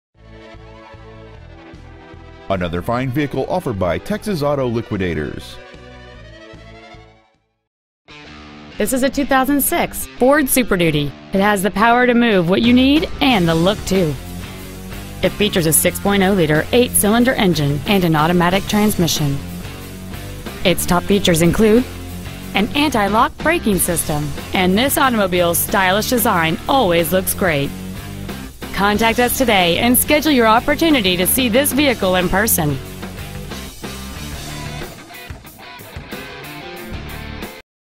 Preowned 2006 Ford Super Duty Arlington TX
Year: 2006
 Make: Ford
 Model: Super Duty
 Engine: 6.0L DI V8 TURBO-DIESEL POWER STROKE ENGINE
 Trans.: Automatic
 Exterior: White
 Miles: 81,819
 Interior: Gray

 Pre-Owned 2006 Ford Super Duty Arlington TX